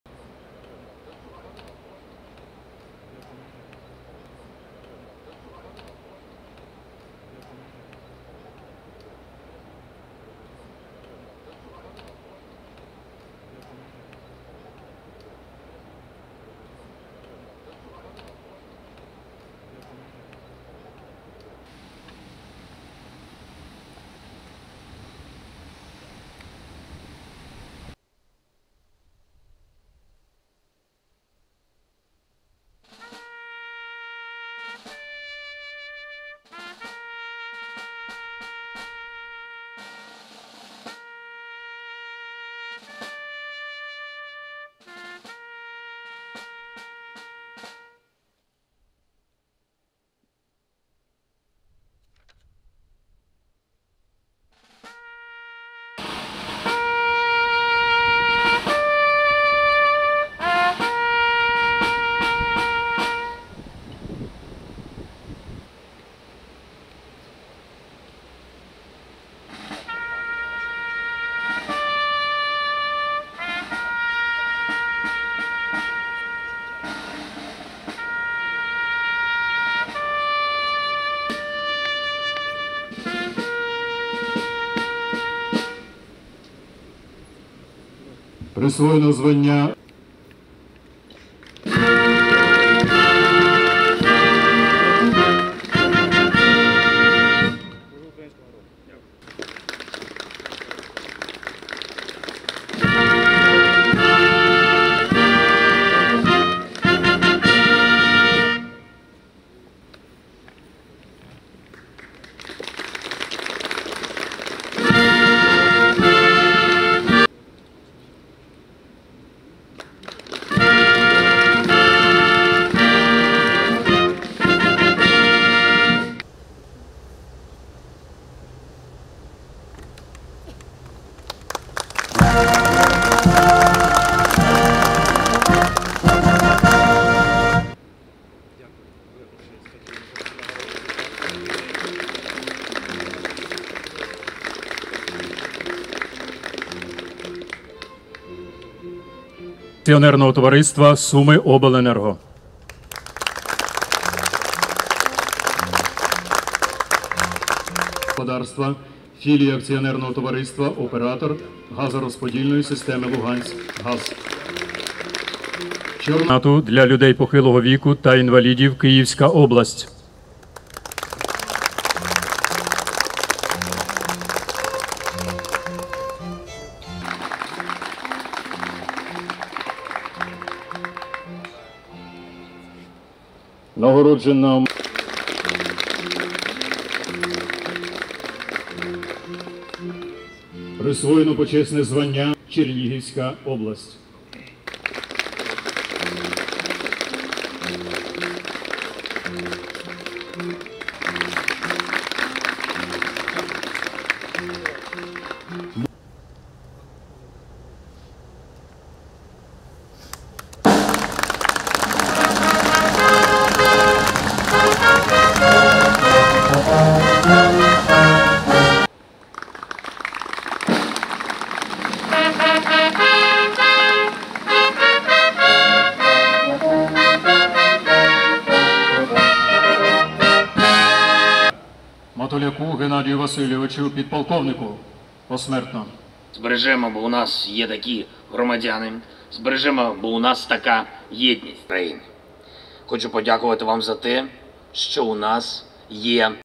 Володимир Зеленський вручив державні нагороди людям, завдяки яким Україна зберігає свою незалежність
Володимир Зеленський вручив високі державні нагороди людям, завдяки яким Україна зберігає свою незалежність

З нагоди Дня Незалежності України Президент Володимир Зеленський вручив ордени «Золота Зірка» військовослужбовцям, яким присвоєно звання Героя України, і членам родин загиблих Героїв, а також інші державні нагороди.